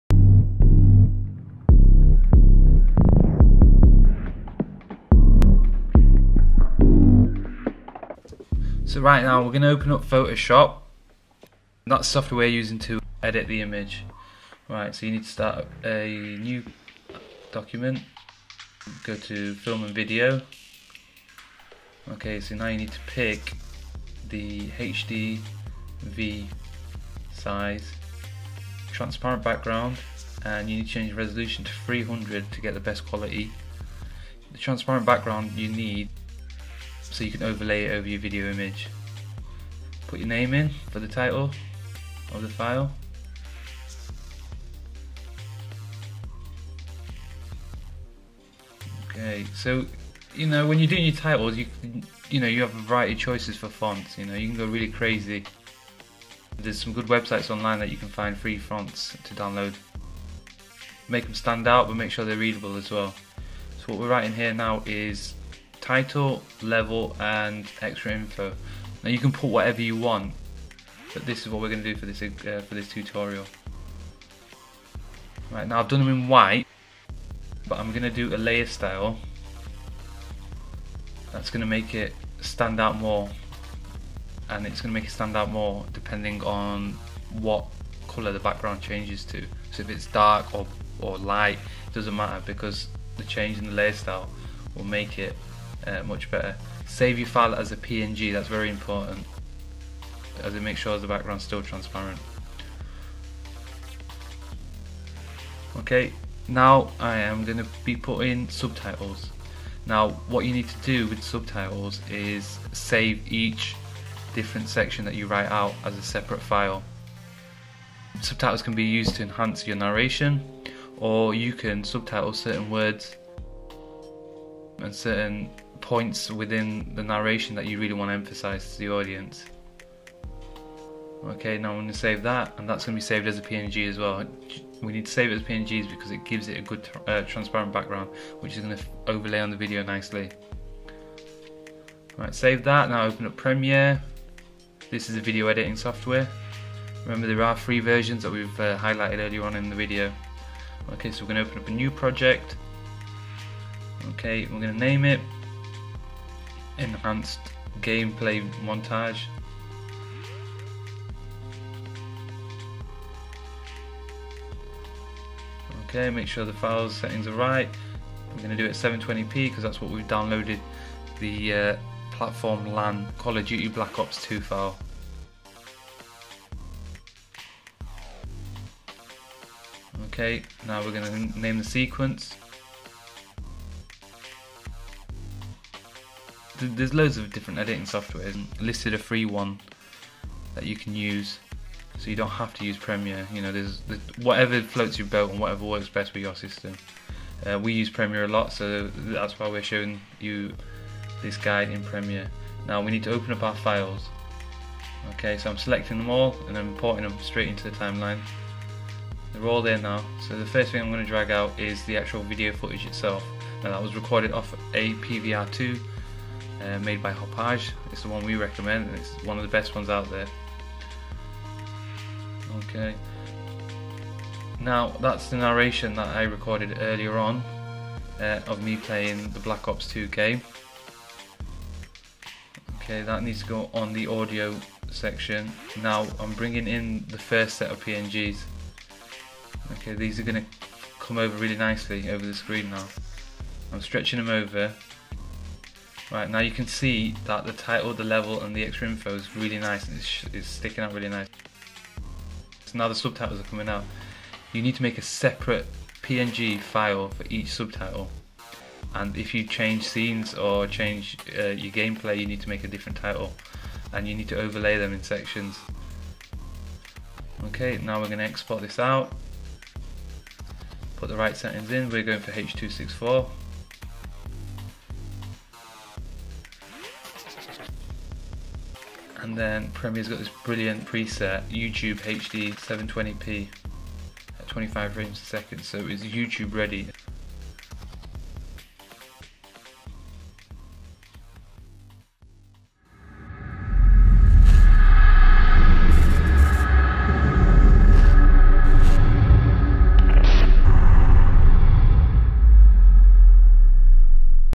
Platform On-Line - How to make an epic title or subtitle set
Ever wanted to know how the Pro's make those epic title screens? Check out our quick fire tutorial by Let There Be Light Productions on just how to get the most out of your videos.

Want some footage to test out your skills, dont worry we have your back there too with our Stock Footage Gallery